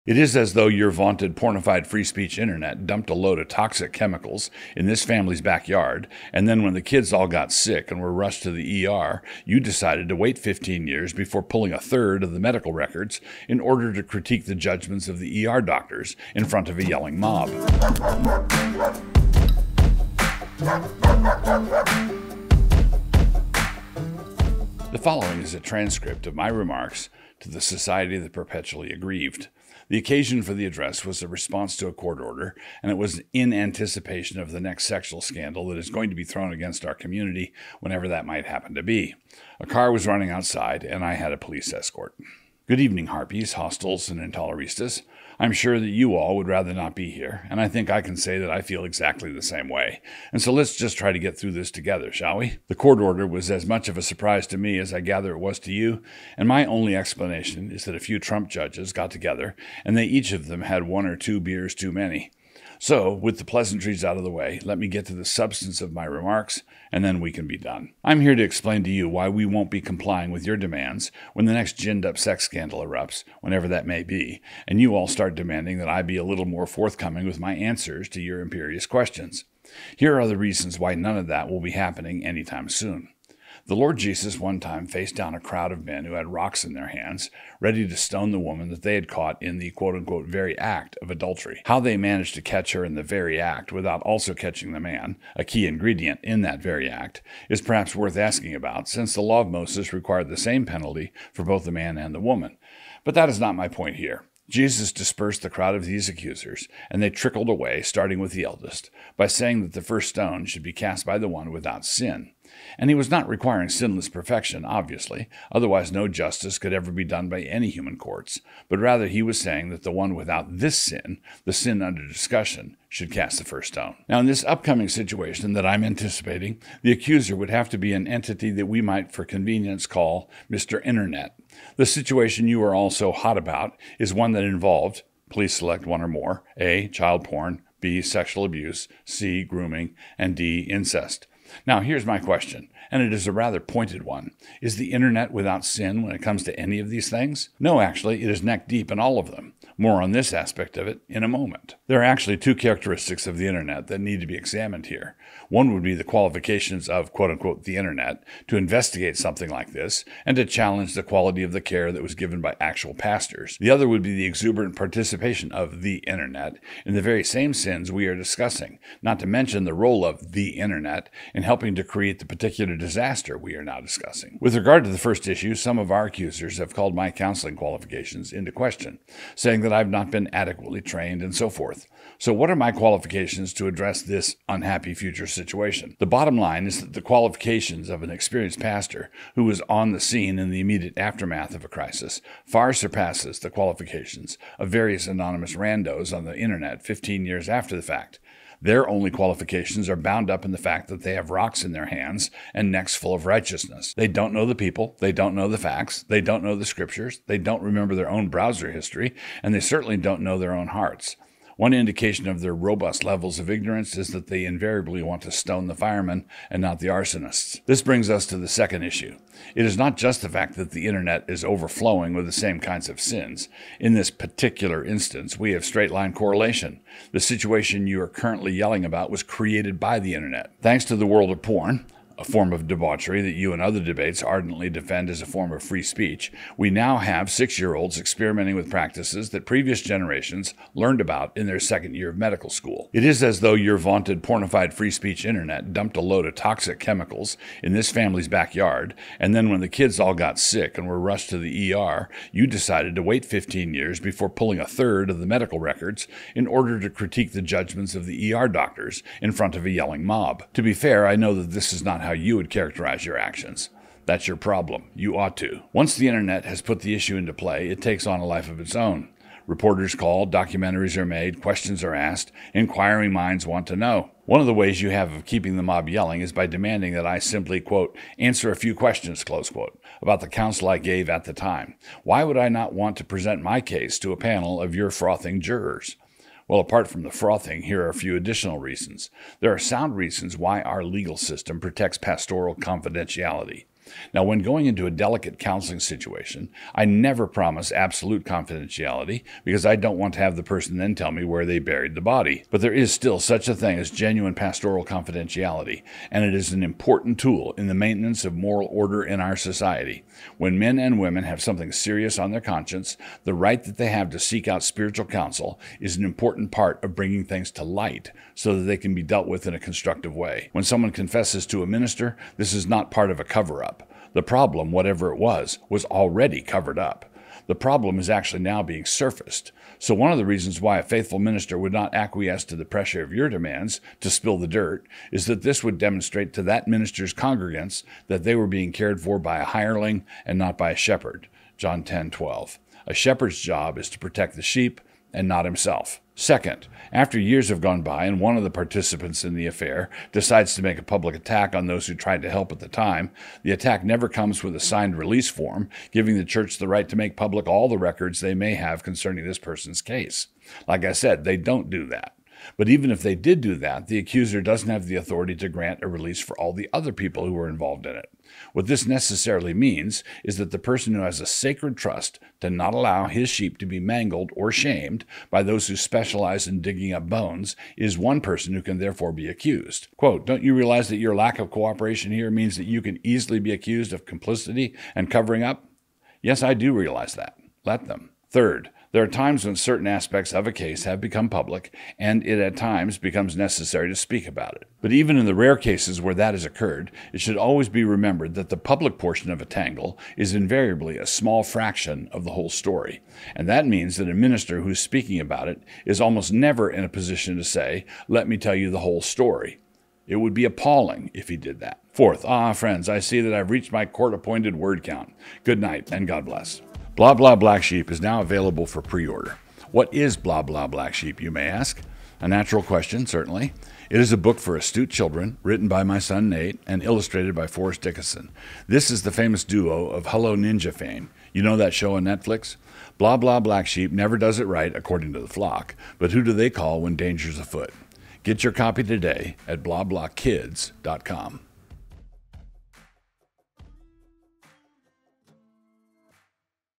As the Internet Is Without Sin, We Will Let It Cast the First Stone | Doug Wilson
The following is a transcript of my remarks to the Society of the Perpetually Aggrieved. The occasion for the address was a response to a court order, and it was in anticipation of the next sexual scandal that is going to be thrown against our community, whenever that might happen to be. A car was running outside, and I had a police escort.

Pastor Doug Wilson's Blog and Mablog video is presented by Canon Press.